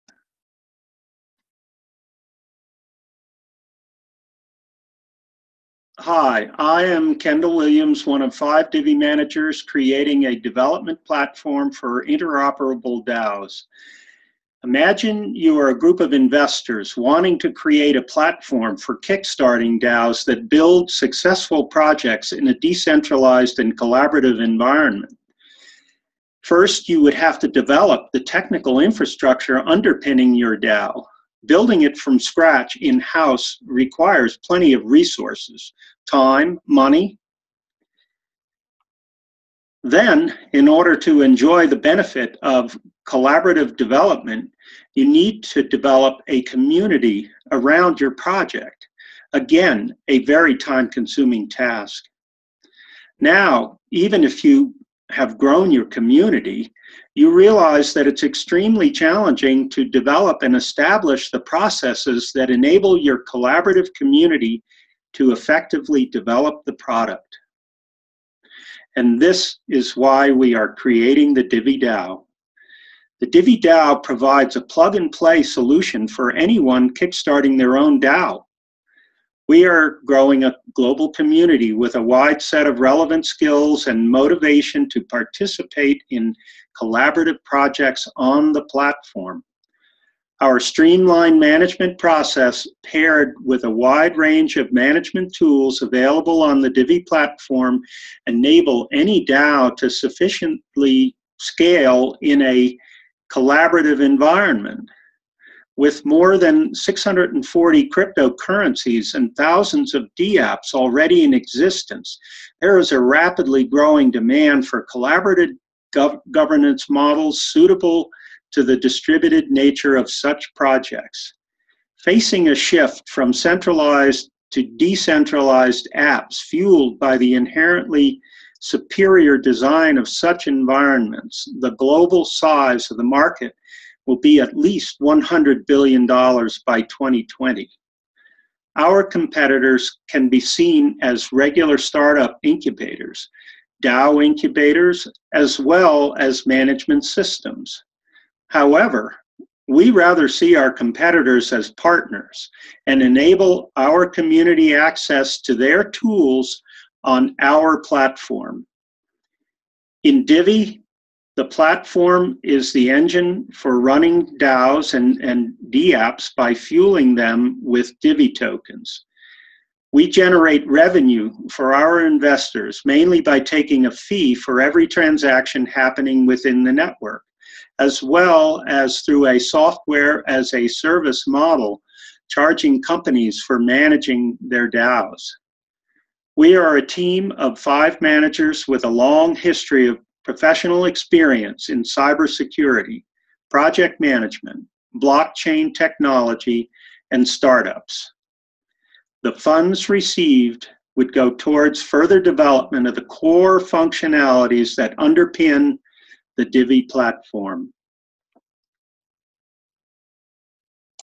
Top 20 - Finalist Pitch: Divvy DAO (Pitch #19)
Divvy DAO creates a platform to create DAO's more effectively and flawlessly.
Presented by Kendall Williams